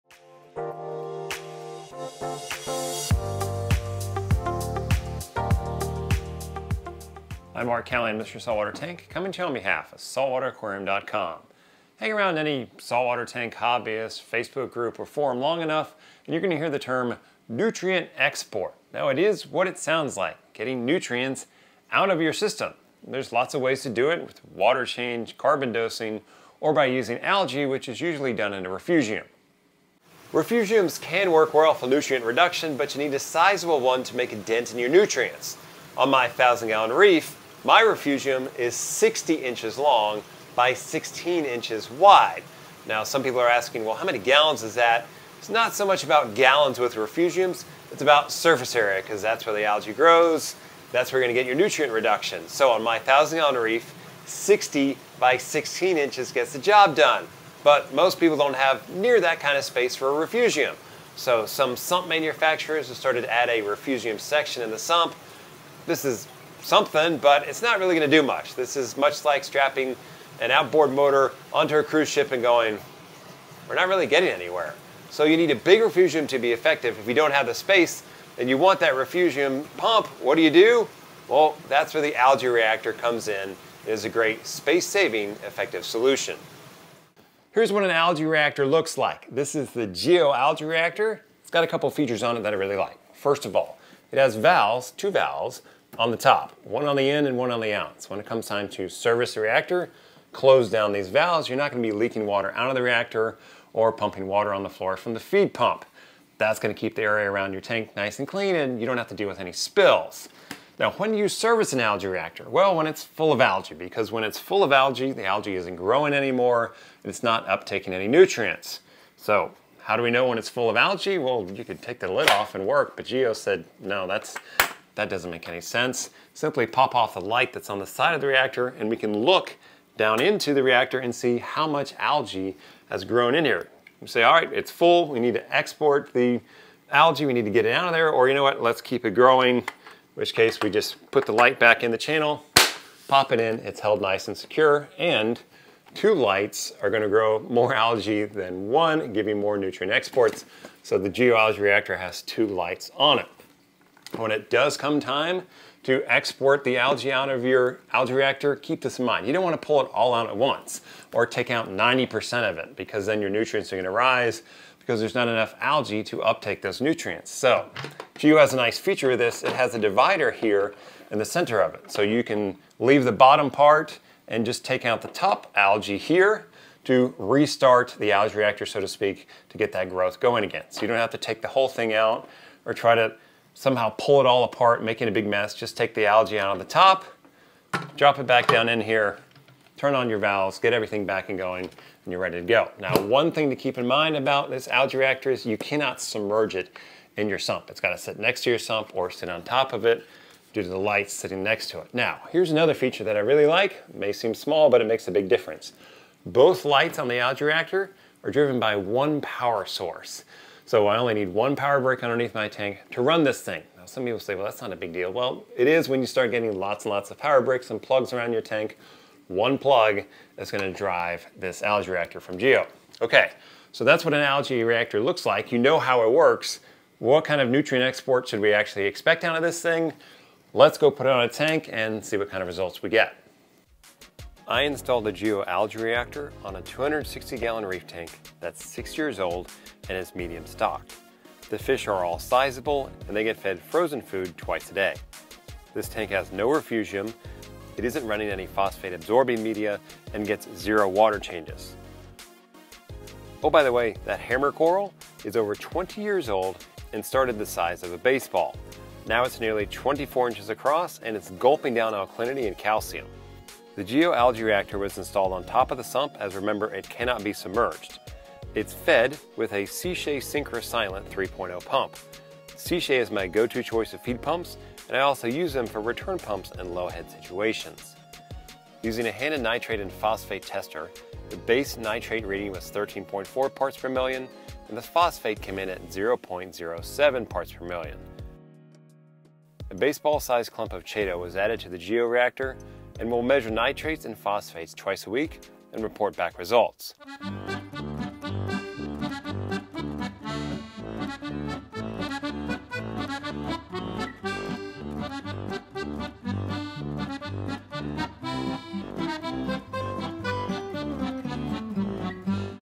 Geo's Reef ALGAE REACTORS - Mr. Saltwater Tank - Raw, Uncut, and First Impressions
In this episode of our series, Mr. Saltwater Tank - Raw, Uncut, and First Impressions (RUF), Mark will check out the Geo's Reef Algae Reactors. Join us as we share how these Algae Reactors are a space saving, effective solution to nutrient exporting. We'll show you how the external LED bar promotes algae growth and how and when to service the reactor for optional use. We'll also share what kind of nutrient export can be expected.